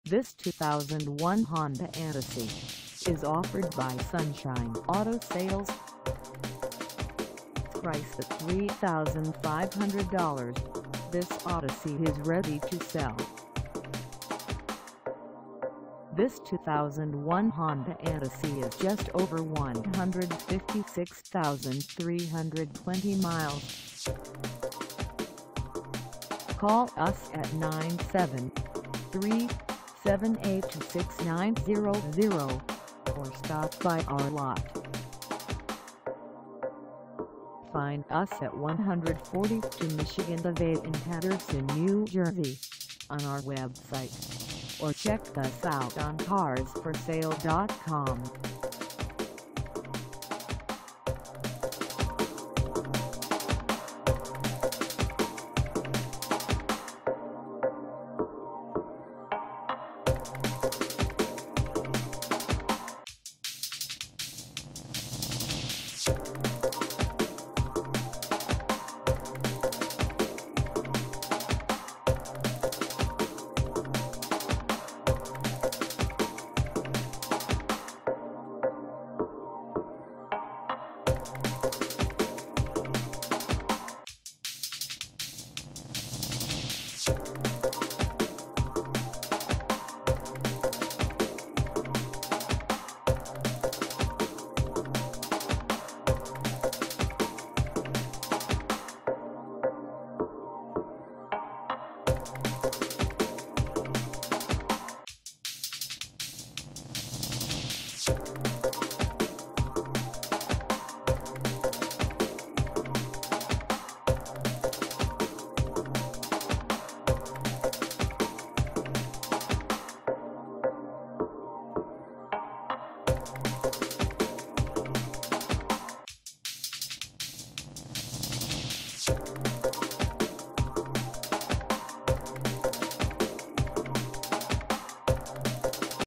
2001 Honda Odyssey EX 4dr PWR DOORS for sale in Paterson, N
This 2001 Honda Odyssey EX 4dr PWR DOORS  is for sale in Paterson, NJ 07503 at SUNSHINE AUTO SALES.
Engine: V6 3.5L V6
Transmission: AUTOMATIC
Color: GRAY
Mileage: 156320
Contact SUNSHINE AUTO SALES 142 MICHIGAN AVE Paterson, NJ 07503 to test drive this 2001 Honda Odyssey EX 4dr PWR DOORS  today.